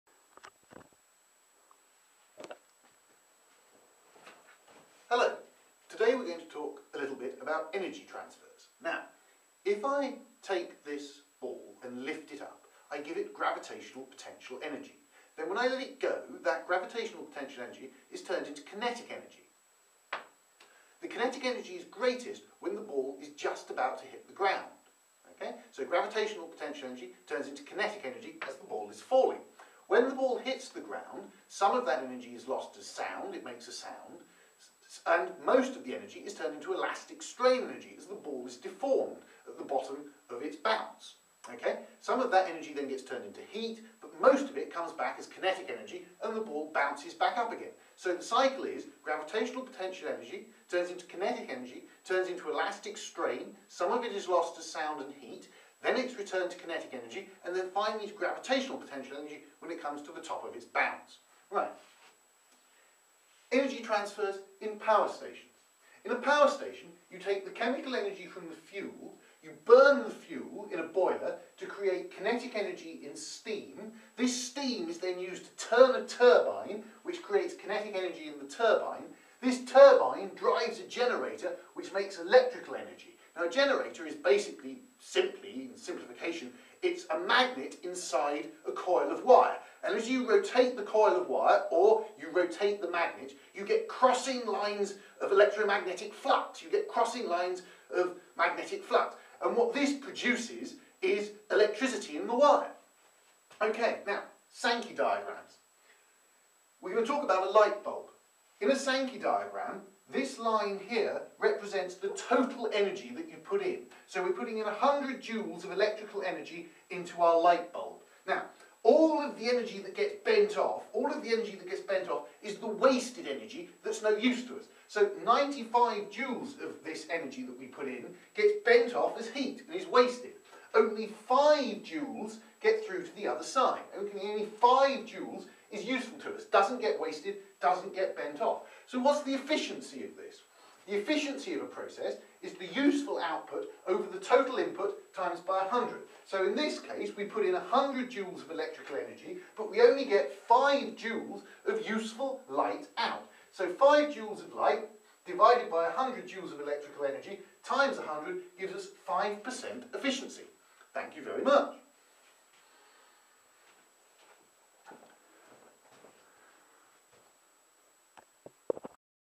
GCSE Physics Part 8 Energy Transfers.MOV
The Principle of Sankey Diagrams. The Equation of Energy Efficiency.